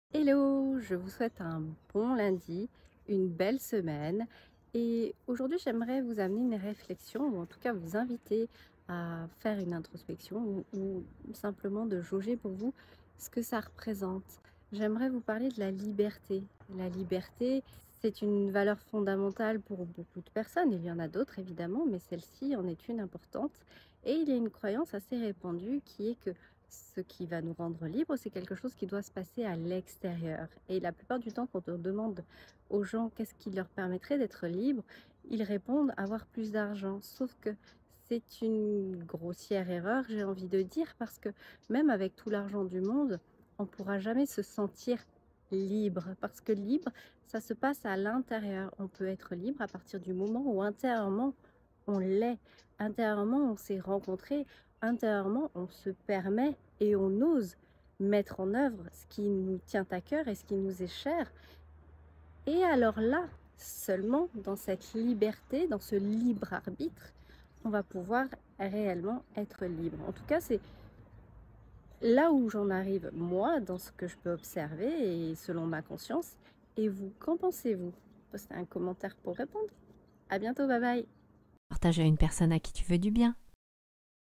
Messagedulundi - La liberté
Si vous affectionnez la valeur liberté, alors ce réel est pour vous.
La liberté est une de mes valeurs fondamentales.

J’observe ainsi la place de la liberté et ma relation à elle sous toutes les coutures.

Je suis aussi très attentive au rapport et liens que les autres peuvent avoir avec elle. Et je déplore que trop souvent, je suis le témoin d’un lien trop surfait entre argent et liberté.
Ou entre richesse et bonheur d’ailleurs

C’est quoi la liberté pour vous ?
Répondez en commentaire ;)